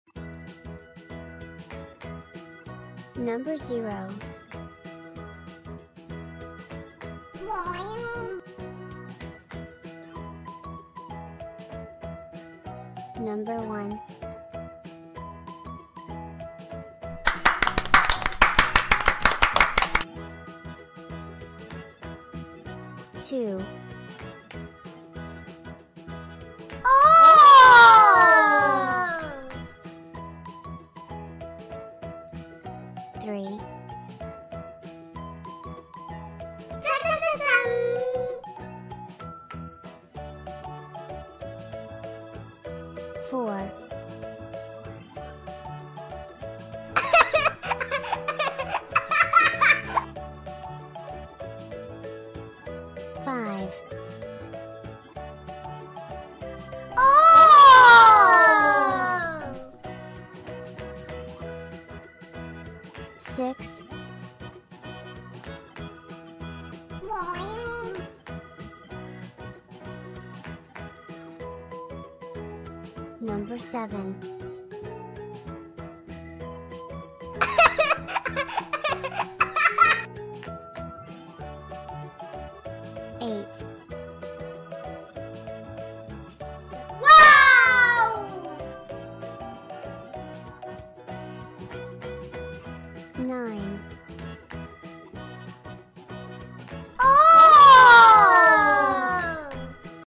Learn numbers for kids | Learn numbers for toddlers
Educational video for children to learn numbers from 0 to 9. Educational video to learn the numbers